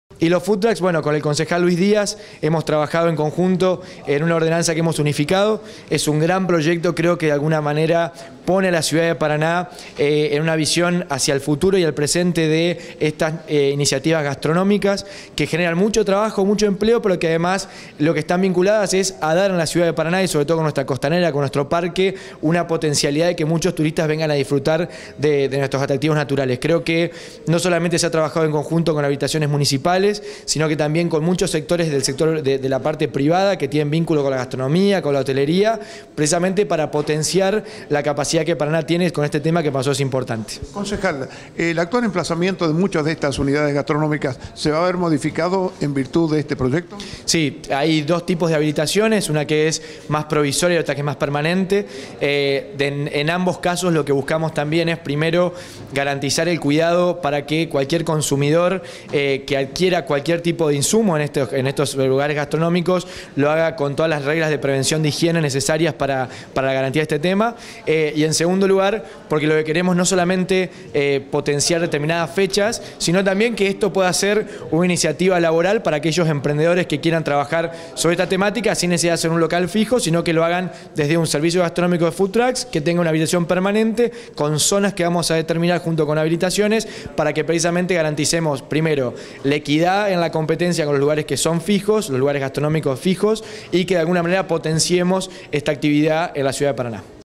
Marco regulatorio para la habilitacion de Food Trucks
Despachos de Comisión
Entre los proyectos que irán al plenario de este viernes con dictamen favorable de las comisiones de Legislación; Salud Pública y de Turismo, Producción y Acción Social figura el que establece el marco regulatorio para la habilitación general y el otorgamiento de permisos particulares de uso precario, para la elaboración y comercialización de alimentos y/o bebidas en los vehículos gastronómicos denominados “Food Trucks”, autoría de los ediles Luis Díaz (Una – Frente Renovador) y Emanuel Gainza (Cambiemos).